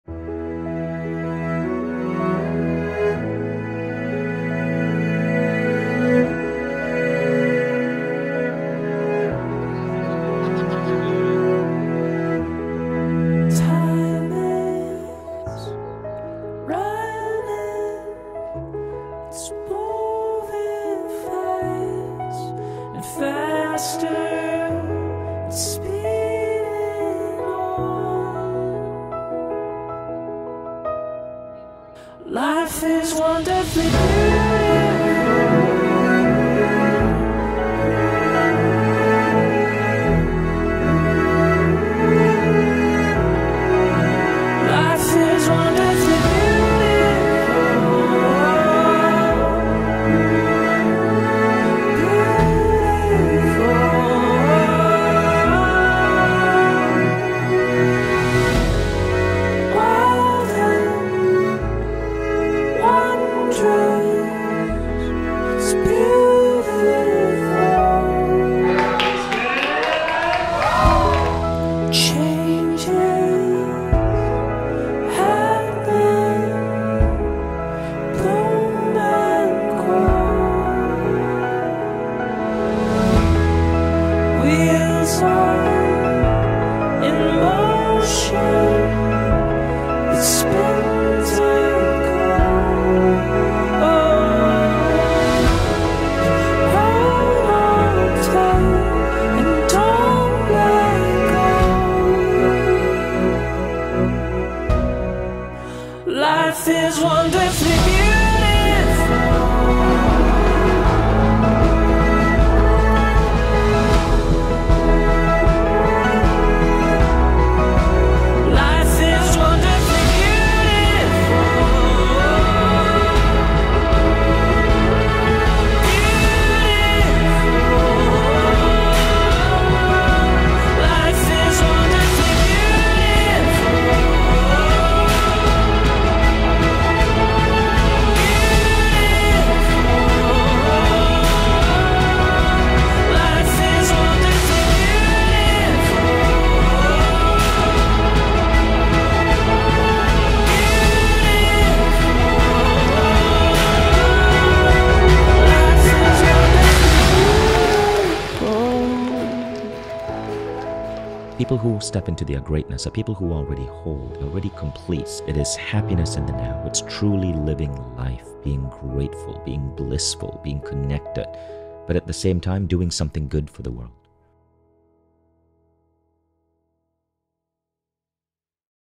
Wonderfully Beautiful Mindvalley University 2022 Closing Ceremony Video
The Closing Ceremony video from Mindvalley University 2022 in Tallinn, Estonia.